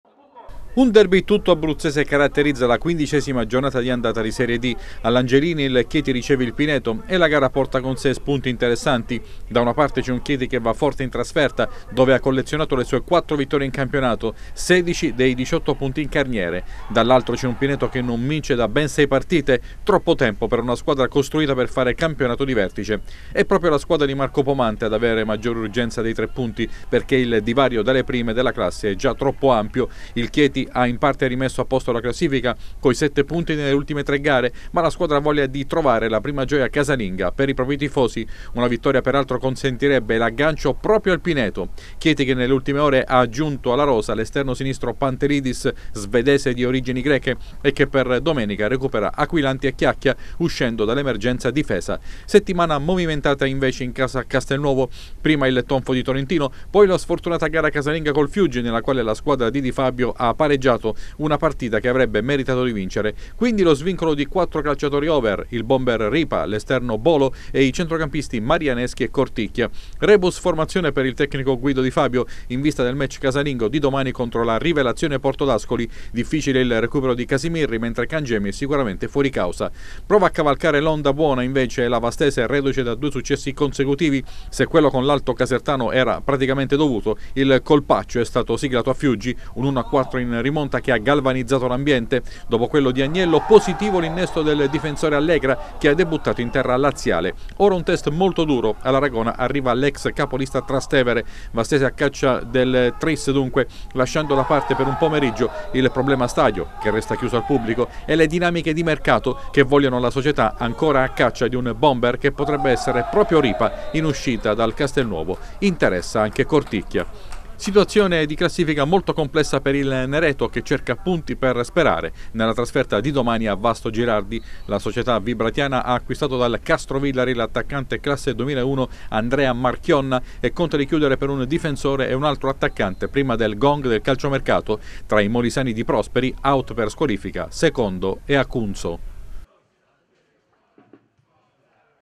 Serie D. Spicca il derby tra Chieti e Pineto
Terzultima di andata nel girone F di Serie D. Il Notaresco anticipa oggi al Savini col Montegiorgio. Domani le altre partite. Fari puntati sull’Angelini dove c’è il derby abruzzese tra Chieti e Pineto. In casa Vastese e Castelnuovo, trasferta molisana per il Nereto